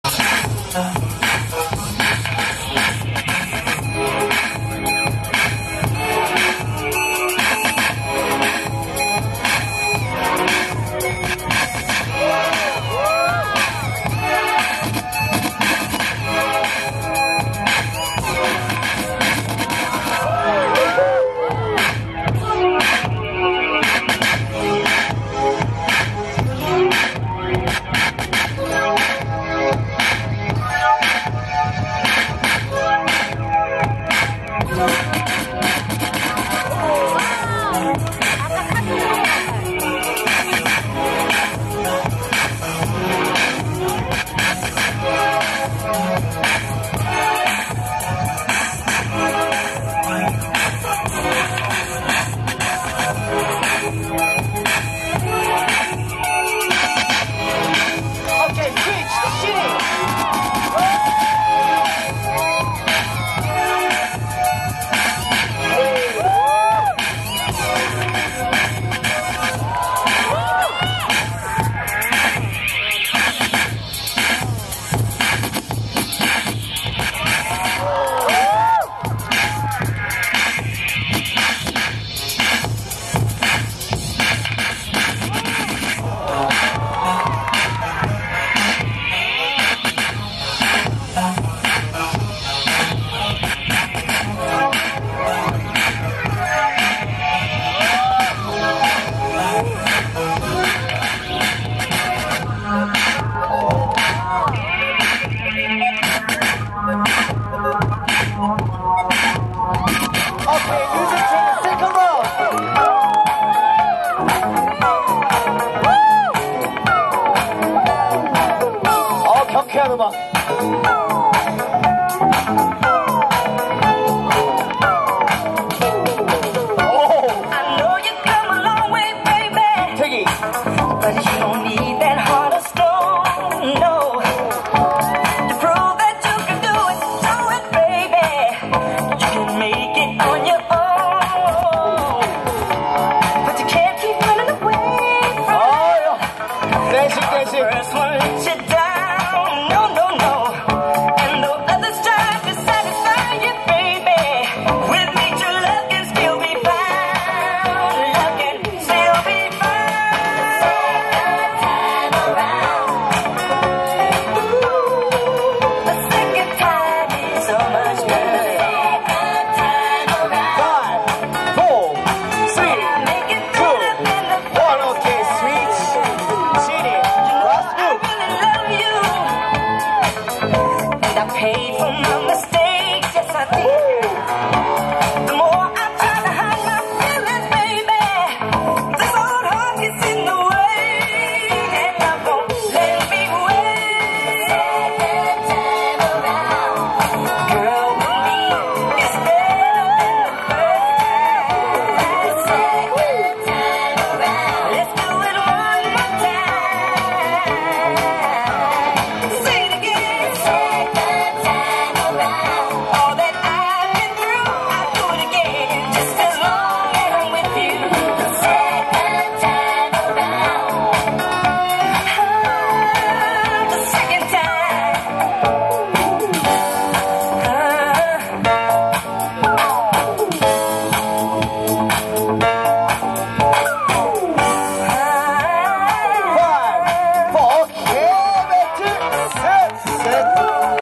All Street Dance 1:1 Battle 頂點(정점) Vol.2 16강 3경기 우진희 vs 김태기
All Street Dance 1:1 Battle 頂點 Vol.2
2012/10/14

16강 3경기 "우진희 vs 김태기"

MC. Du Lock (from. funky street prince)
DJ. 5000 (from. love & peace)

Judge.
PopKun (from. brand new mind)
Kimil (from. brooklyn monkeyz)
Soul K (from. ghost of soul)

Guest Show.
Cube Sound

Presented by 'EARTHQUAKE'